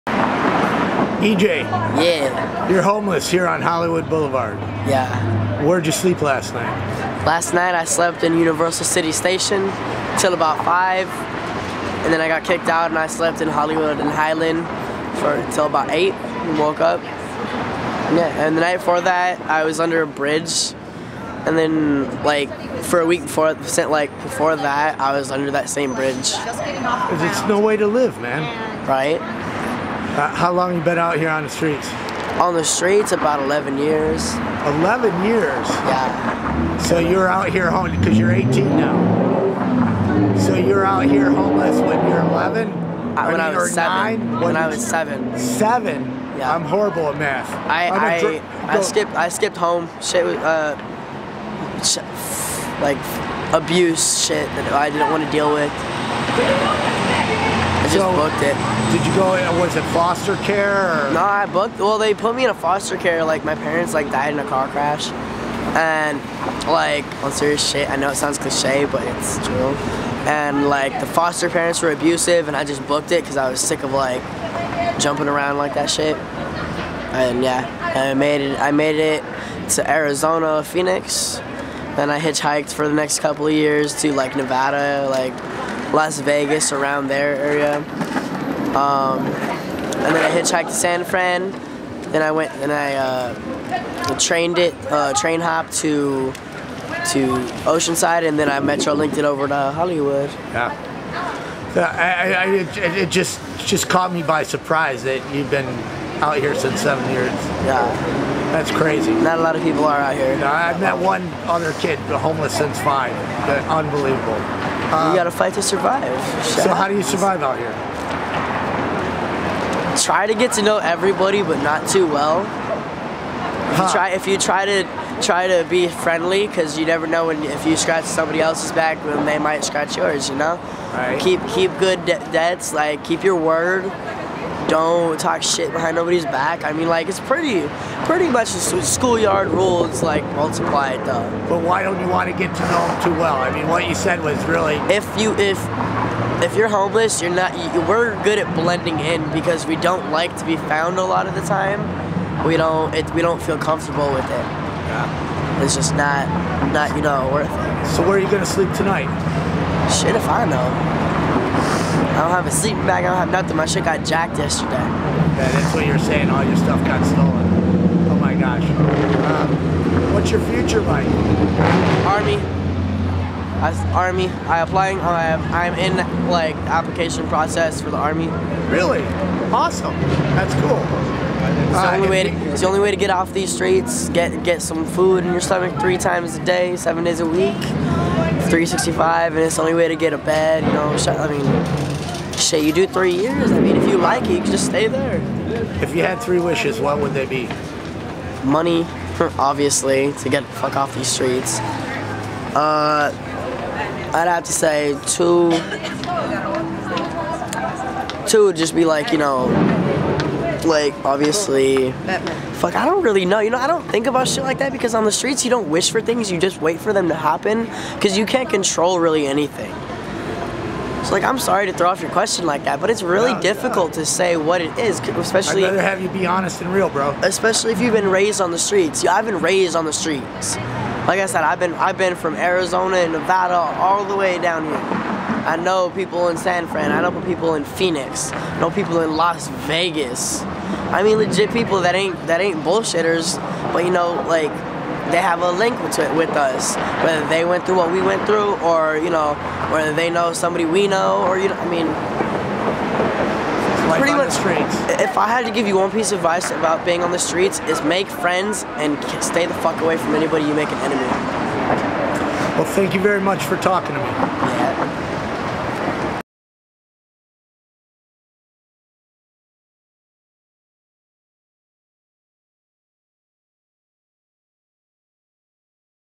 EJ is 18. He told me he has been homeless for 11 years.
"On the streets, you don't wish for things. You just wait for them to happen. Because you cannot control really anything", EJ responded trying to think of a second and third wish.

EJ is homeless in Hollywood. He is 18, and when he told me he has been homeless for 11 years, I could not even comprehend to do the math. (I'm a drummer and drummers can't count past 4 anyway)

I cannot even imagine being on the streets at such a young age. I once interviewed a kid in Wichita homeless at 5 years old I know how horrible my first day homeless was as an adult, so my heart breaks thinking about what these kids must have done through. But to them, the streets were better than the abuse they were going through.

We will never end homelessness until we can fix families!